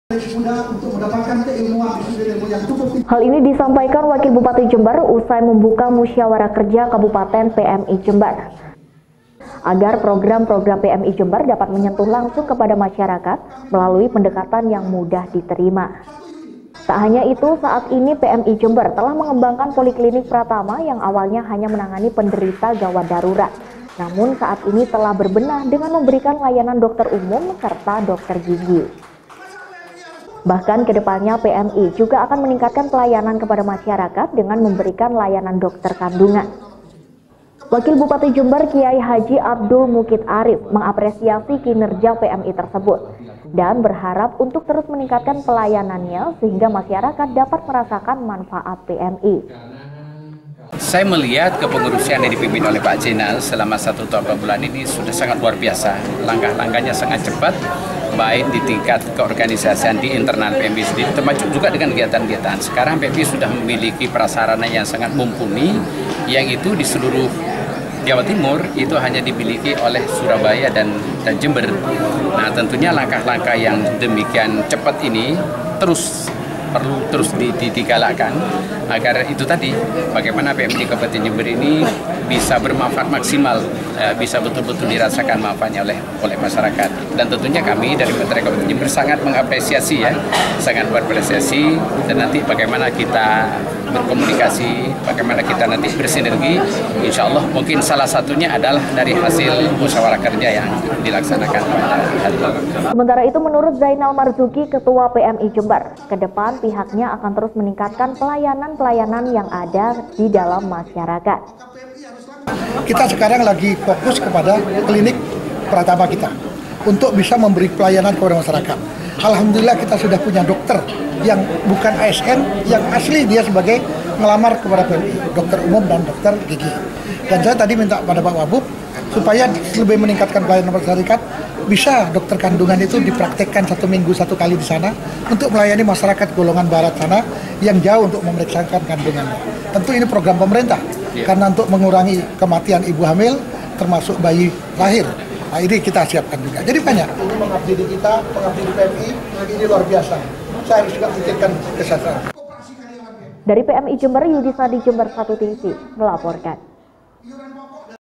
PMI Tingkatkan Pelayanan kepada Masyarakat
Wakil Bupati Jember secara resmi membuka musyawarah kerja palang merah indonesia kabupaten jember, dalam musker ini wabup berharap, PMI dapat melahirkan keputusan yang dapat bermanfaat lebih  untuk masyarakat Kabupaten Jember.